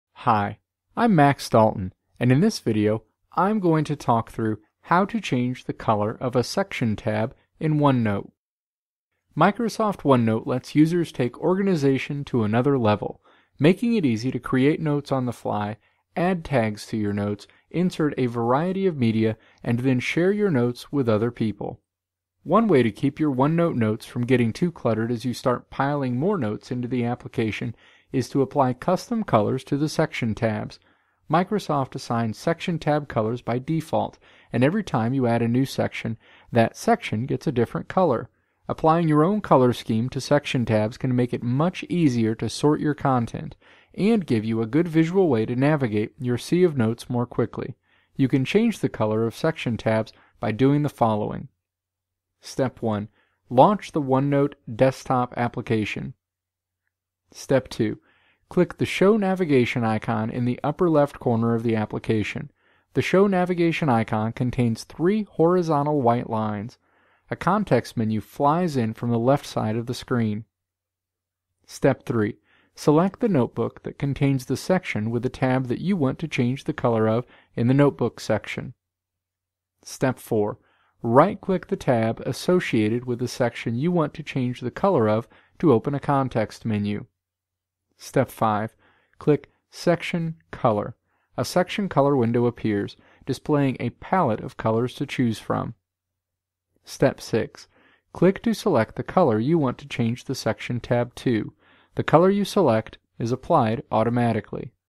How to Change Microsoft OneNote Section Tab Color
This video walks you through how to change the color of a section tab in Microsoft OneNote.